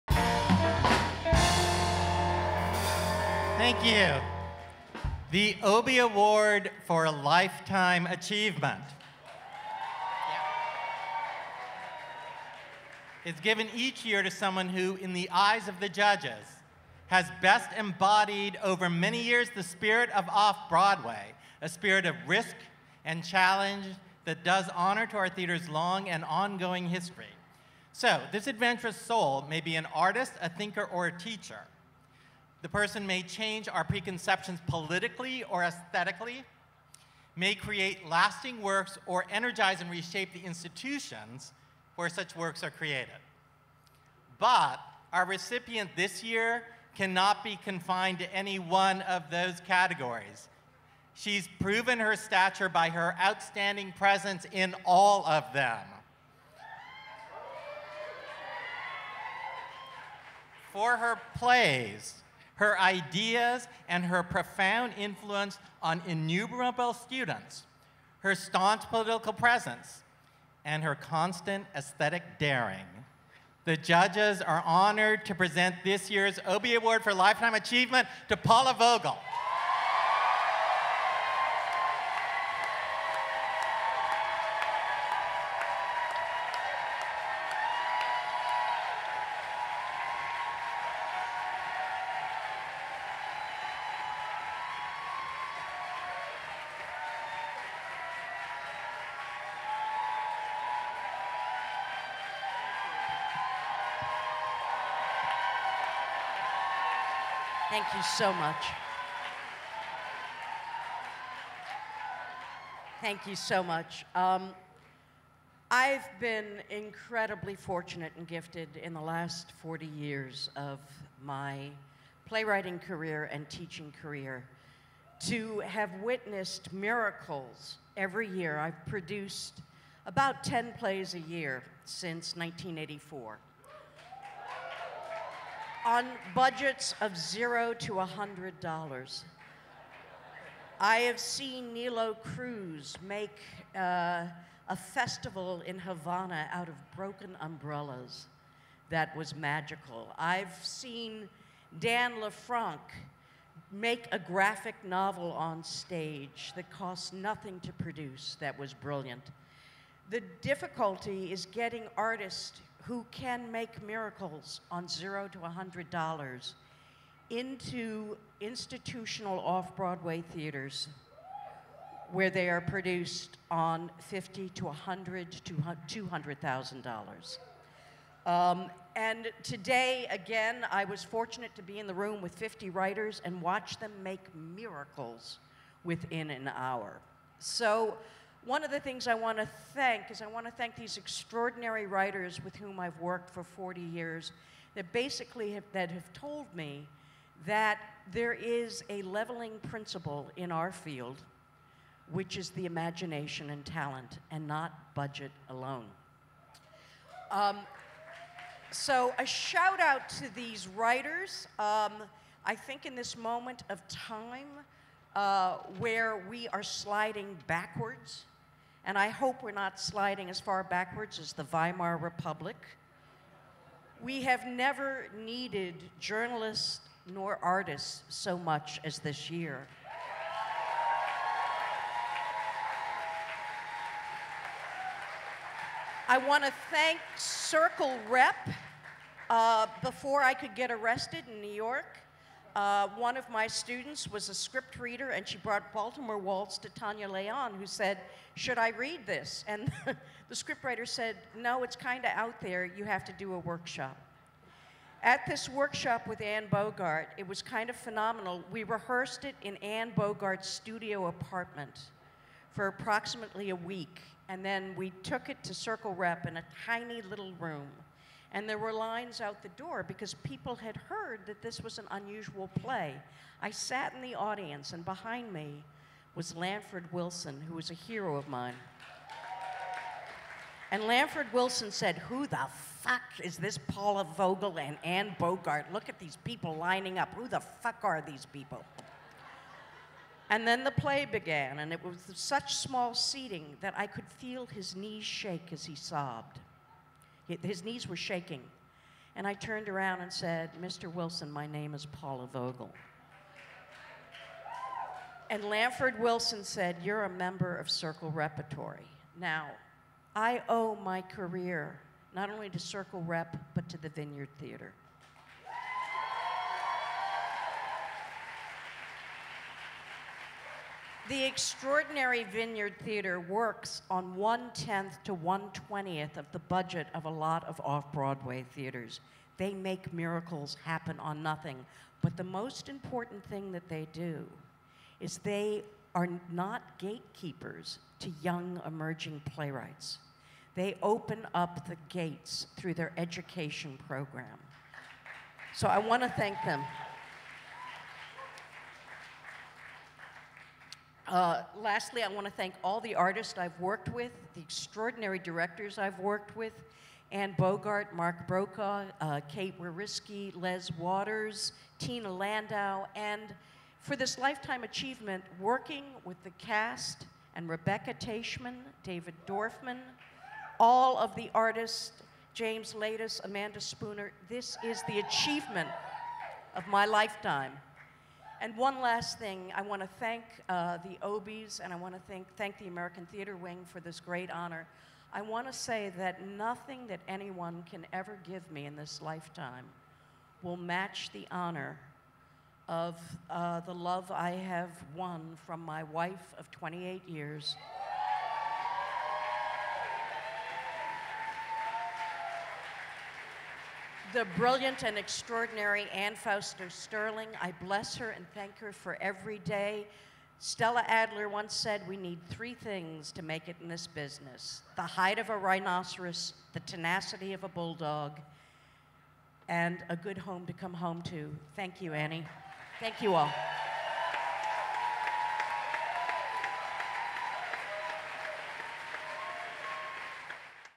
Acceptance Speech: Paula Vogel (2017)
The Obie Award for Lifetime Achievement is given each year to someone who, in the eyes of the judges, has best embodied over many years the spirit of Off-Broadway - a spirit of risk and challenge that does honor to our theatre's long and ongoing history of risk and challenge. This adventurous soul may be an artist, a thinker, or a teacher; may challenge our preconceptions politically or aesthetically; may create lasting works or energize and reshape the institutions where such works are created. Our recipient this year cannot be confined to any one of those categories: She has proven her stature by her outstanding presence in all of them. For her plays, her ideas, and her profound influence on innumerable students, her staunch political presence and her constant aesthetic daring, the judges are honored to present this year's Obie Award for Lifetime Achievement to PAULA VOGEL.